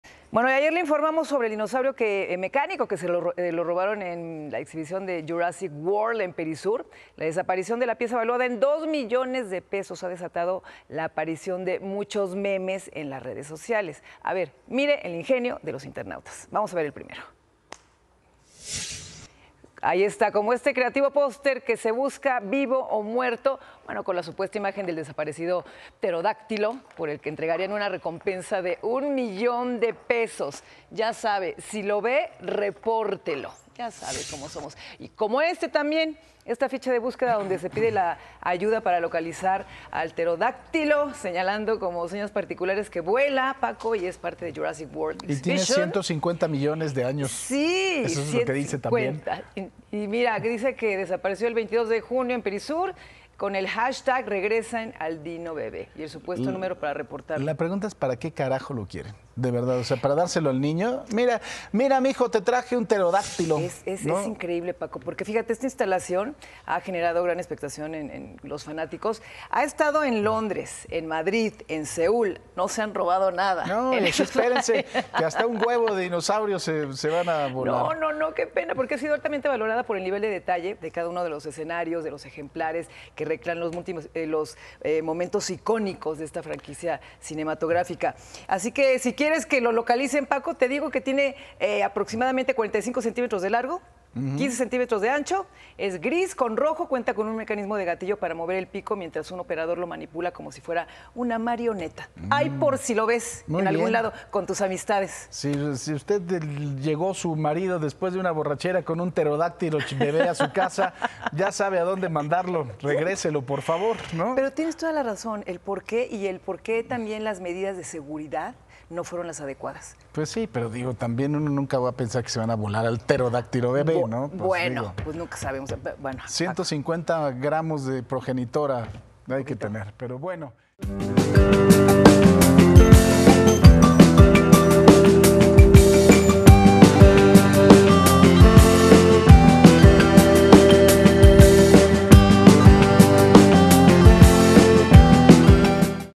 Robo de dinosaurio en exposición de Perisur desata ingeniosos memes
La Fiscalía de la Ciudad de México inició una carpeta de investigación por el robo de un dinosaurio mecánico que fue sustraído de una exposición que se instaló en el centro comercial Perisur. 26 julio de 2024